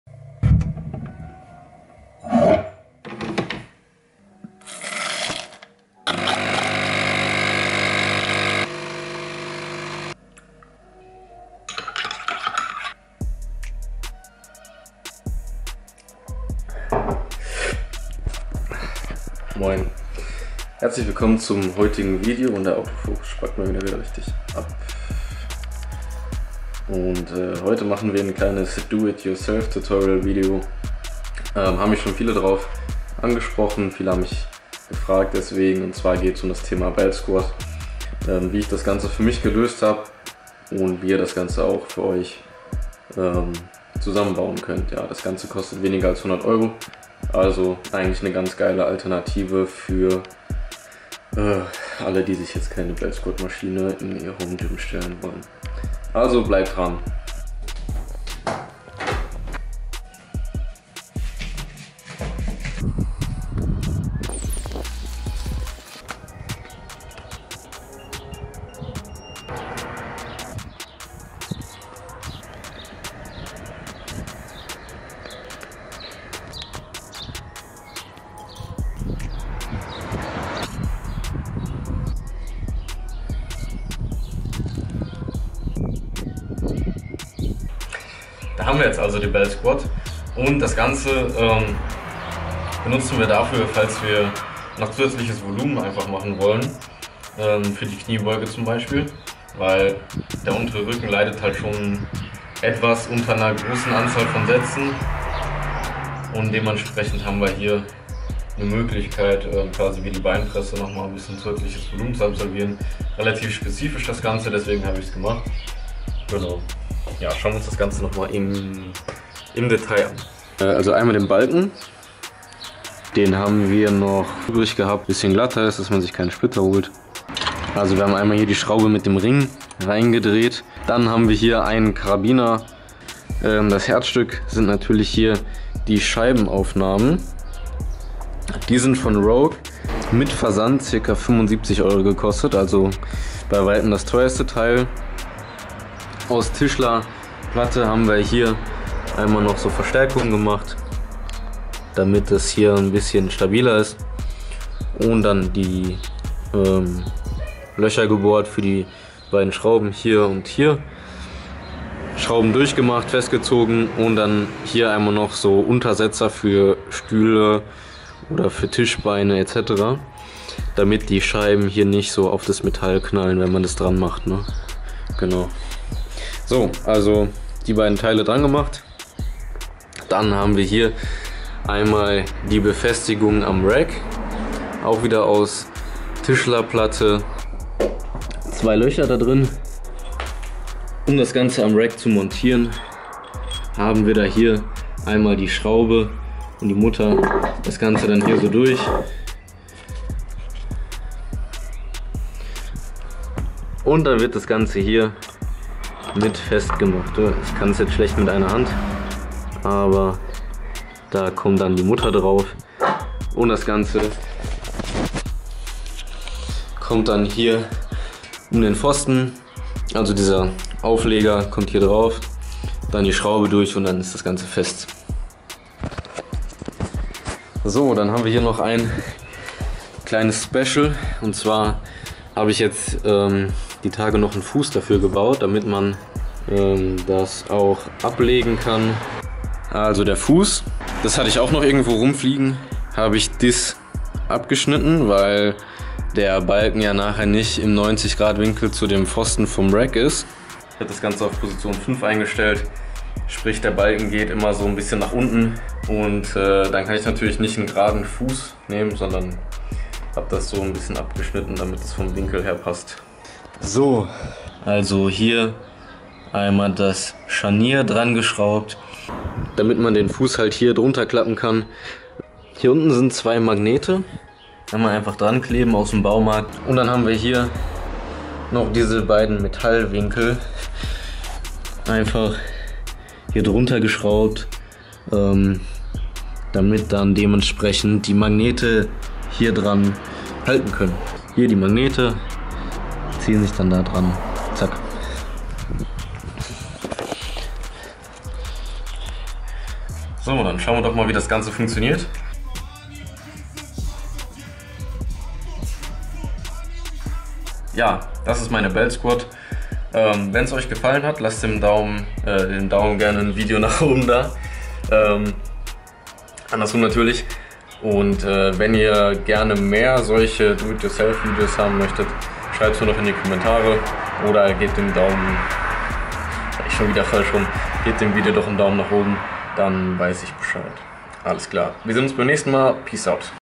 Beltsquat DIY für unter 100€
Heute erzähle ich euch, wie ich mein Beltsquat Attachment gebaut habe!